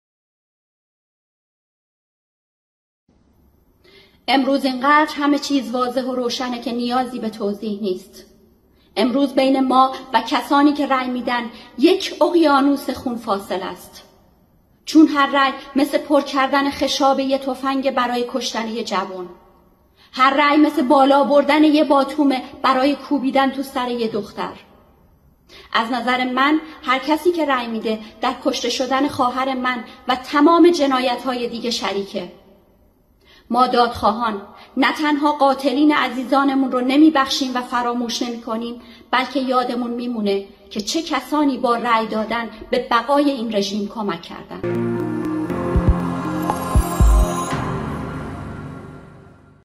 عظمت اژدری: هر رای پر کردن یک خشاب برای کشتن جوانان ایران است
عظمت اژدری، از خانواده‌های دادخواه پرواز پی‌اس۷۵۲: هر رای پر کردن یک خشاب برای کشتن جوانان ایران است